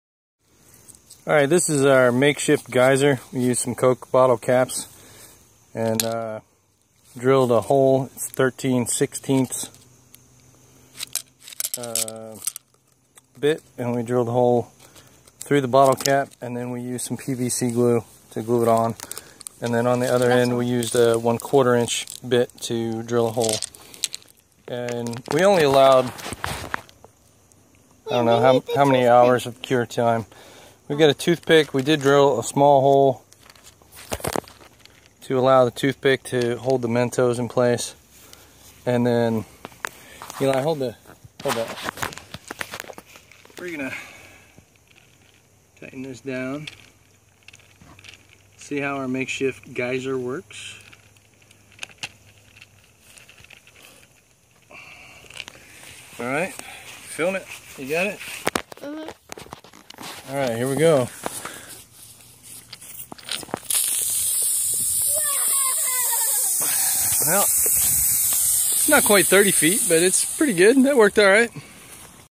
Soda Bottle Geyser - do-it-yourself part 2
How to make your own soda bottle geyser. You'll need...
Six inches of 3/4" pvc, two soda bottle caps, drill, 13/16" bit, 1/4" bit, 1/8" bit, tooth-pic, pvc glue...also, a two letter soda and a fresh pack of Mentos